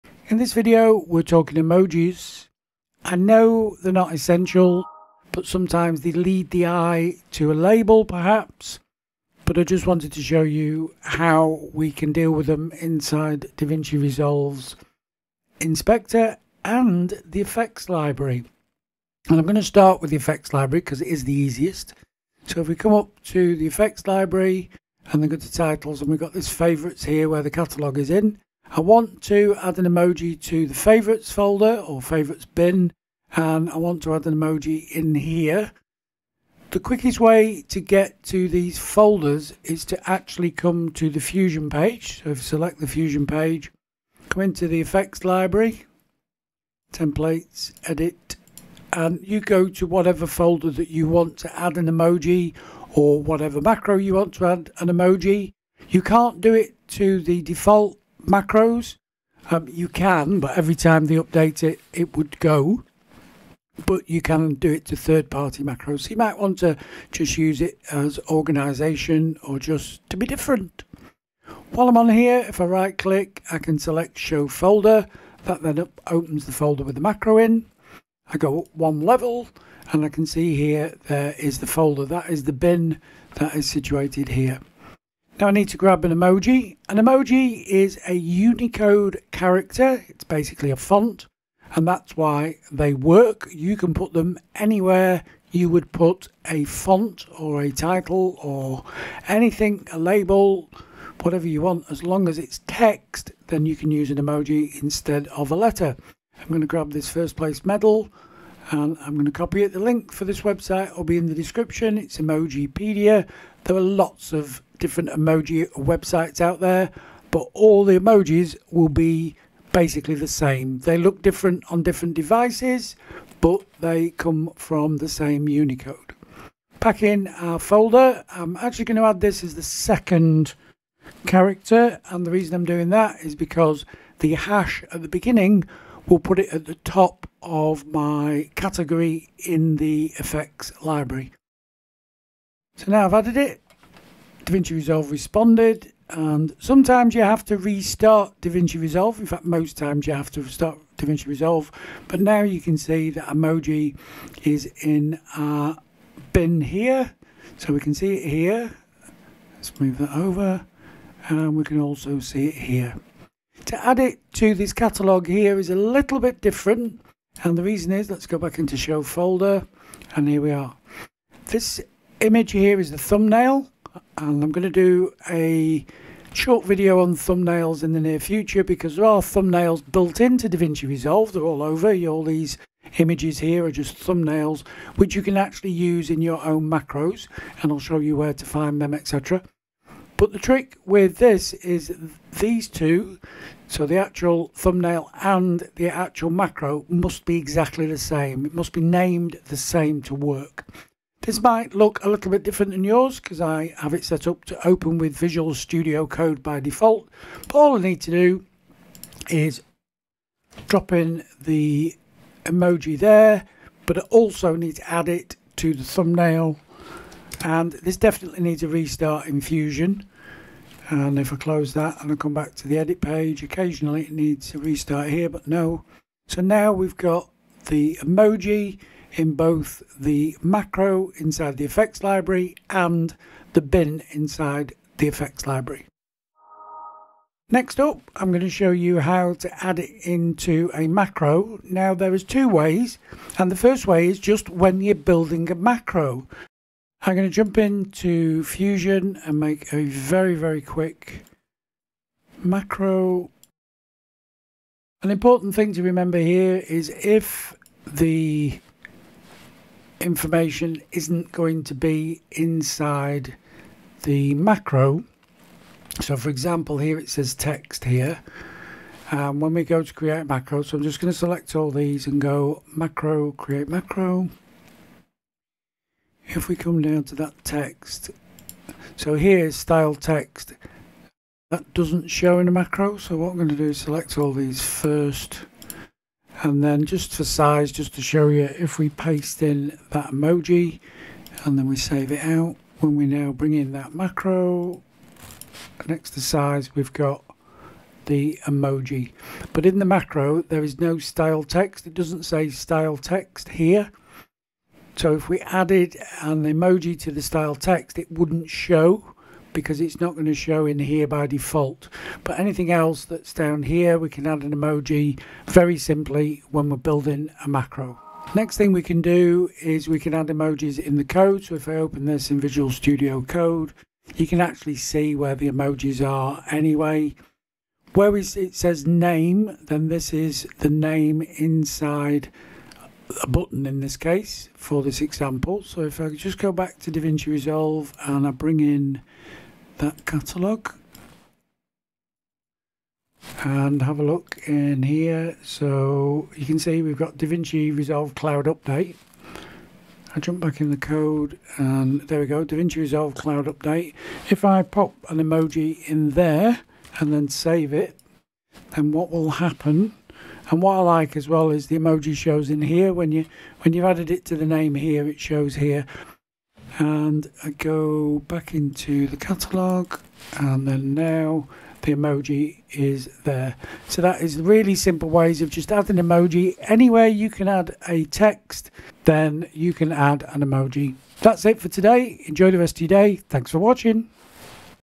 Adding Emojis to Effects and Macros
I know this is a vanity video as there is no real need for Emoji's inside macros or the effects library but it's fun to do. I think they may have a small place to guide users to certain areas. I have started using them in Labels. I have a compass 🧭 on position and a Bright Button 🔆 on levels, I am sure there are lots of use cases. Drop a comment if you use them or just think they are a daft idea.